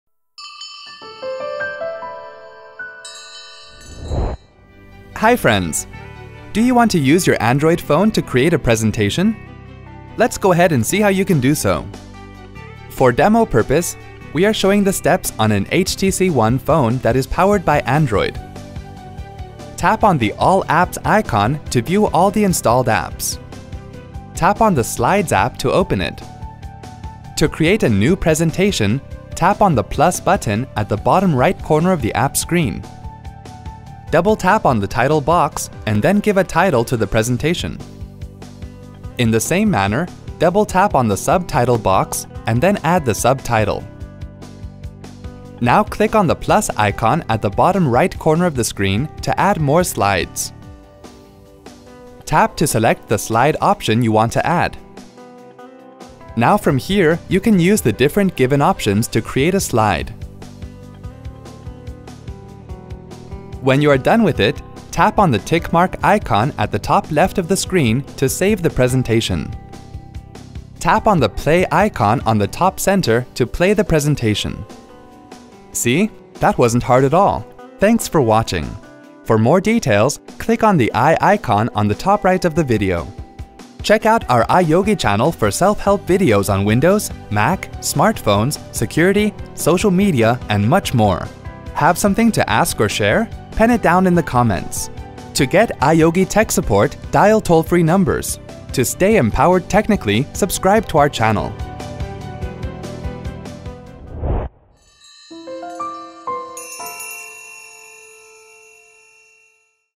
How to create presentations on Android™ phone :Tutorial
If you want to create presentations on Android™ phone, watch this video and follow the steps to do so.